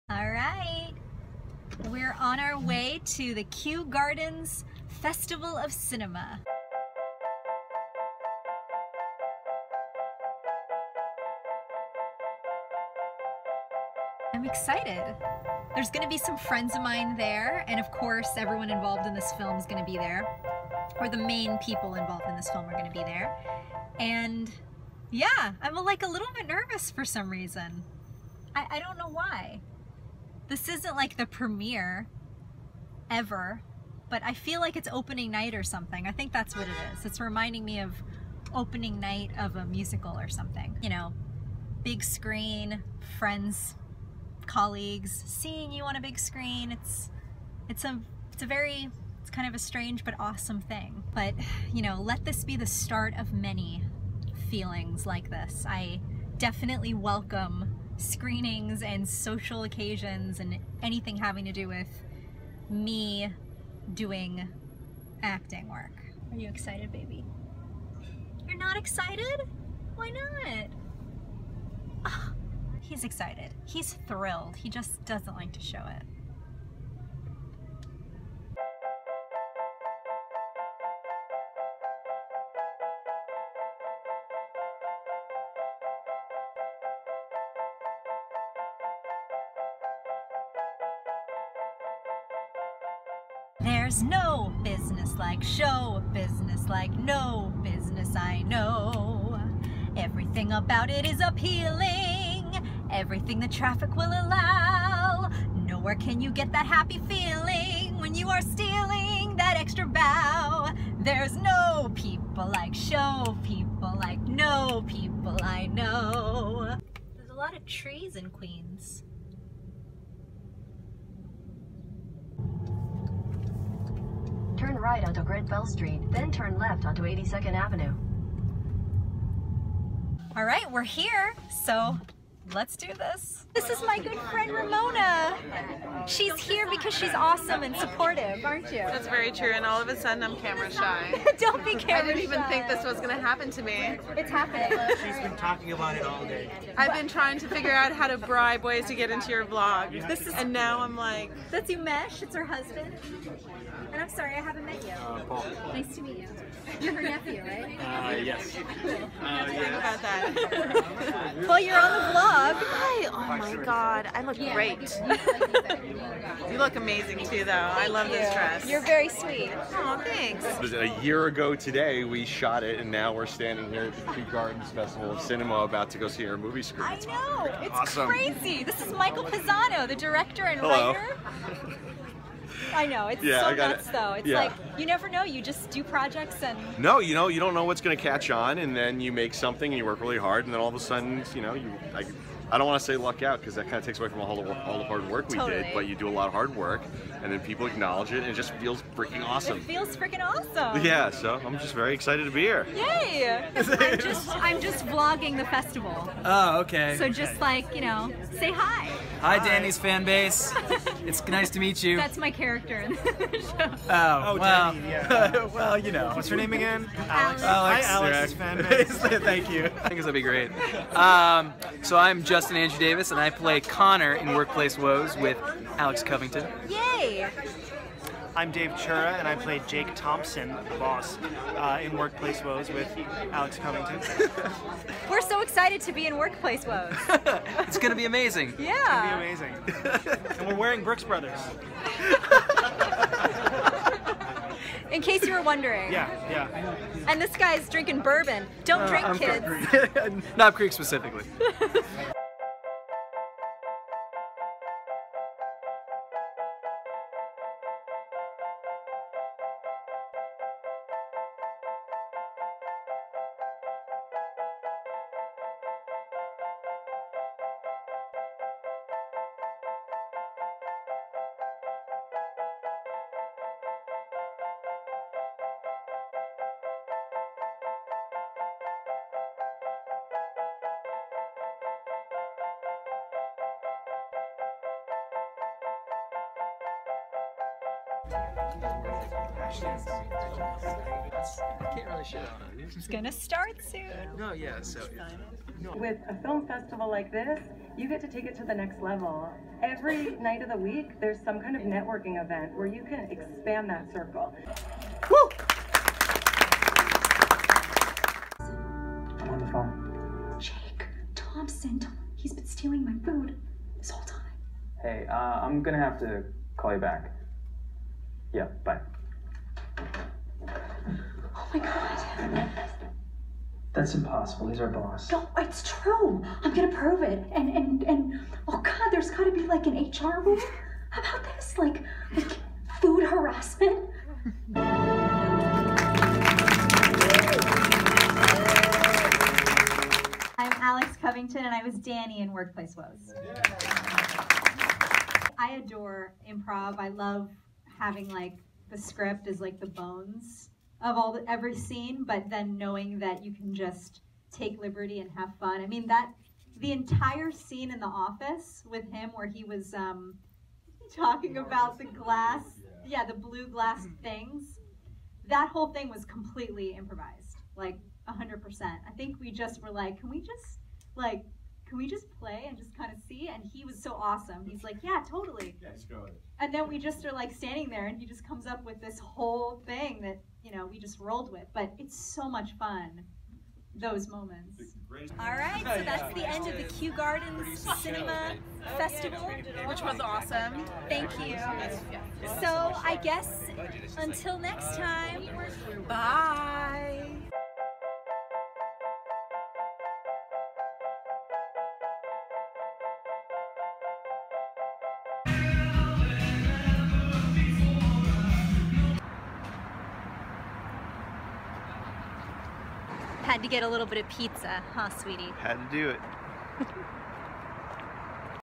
Kew Gardens Festival of Cinema
Vlog #25
"Workplace Woes" A Film we shot last year, has been hitting the festival circuit. In this Vlog, I take you to the Kew Gardens Festival of Cinema! We had a fantastic night :) Happy to have you there!

And if you like this video, hit the "like" button.